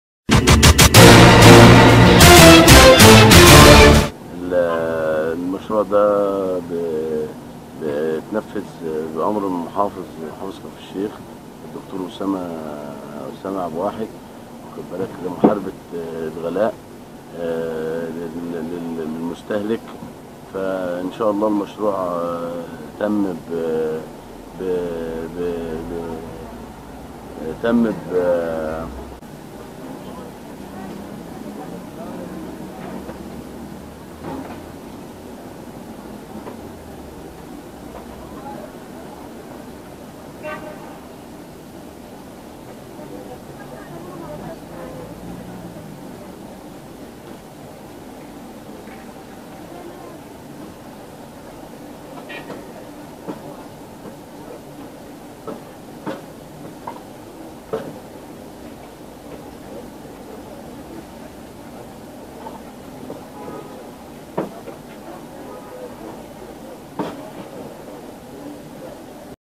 كفر الشيخ تُحارب الغلاء بالمجمعات الاستهلاكية
أنشأت محافظة كفرالشيخ تجمعًا كبيرًا لبيع الخضروات والفاكهة، والسلع الغذائية أمام مجلس مدينة كفرالشيخ وذلك تشجيعًا لتشغيل الشباب، بالإضافة لتوفير جميع الخضروات والسلع أمام المواطنين بسعر مناسب ولمحاربة غلاء الأسعار.

وأشاد حمادة مختار "أحد أصحاب المشروعات التجارية" بفتح منافذ لبيع الخضروات والفاكهة، مؤكدًا أن قرار الدكتور أسامة حمدى، محافظ كفر الشيخ بفتح تلك المجمعات الاستهلاكية فى الأماكن العامة جعل المواطنين لديهم ارتياحا كبيرا لحركة البيع والشراء، بما يضمن لهم شراء السلع بأسعار مناسبة دون زيادة فى الأسعار، مُضيفَا أن المحافظ لديه رؤية واضحة لمحاربة الغلاء، وتوفير السلع بإنشاء منافذ جديدة بمدن المحافظة وقُراها، وخلق فرص عمل حقيقية لتشغيل الشباب.